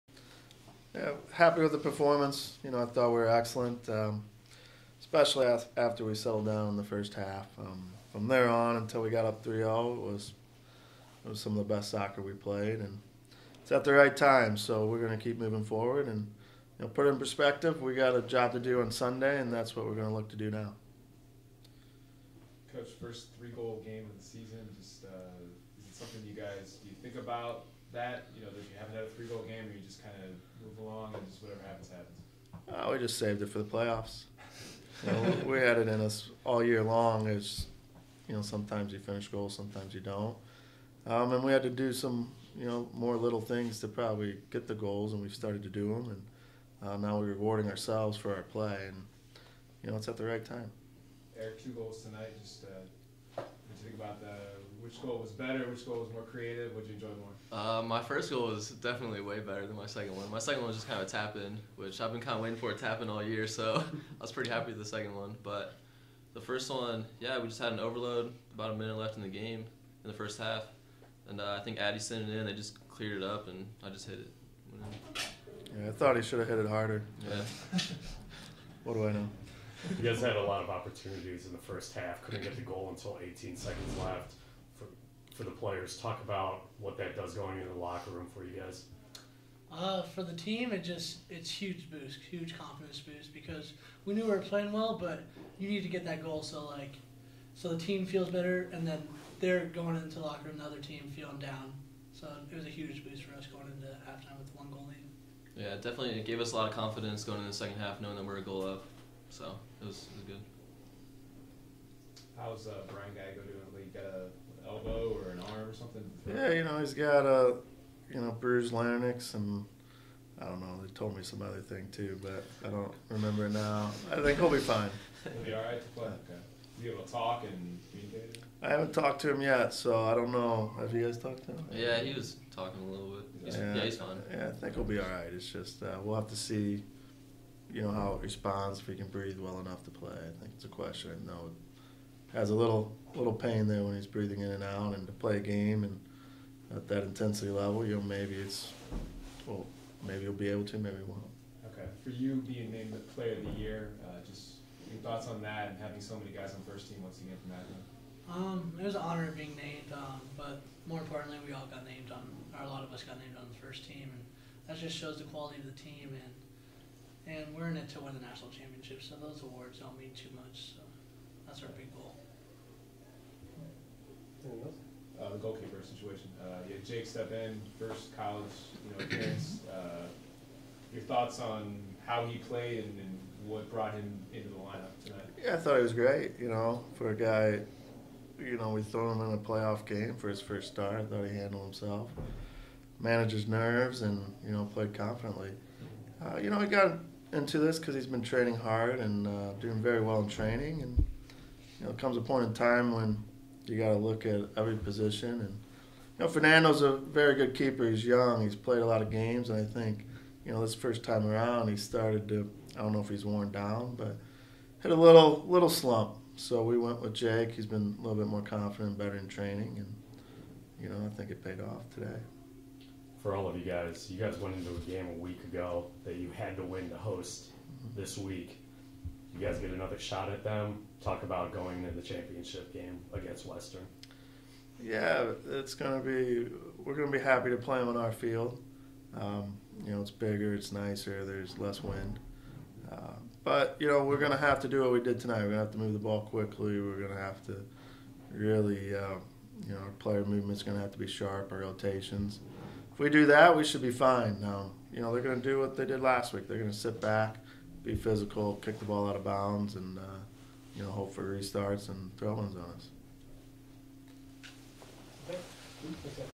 MAC Men's Soccer Championship Game 2 Postgame: Akron Zips
Akron head coach Jared Embick and goal scorers Aodhan Quinn and Eric Stevenson address the media following the Zips' 3-0  win over West Virginia in the MAC Semifinals.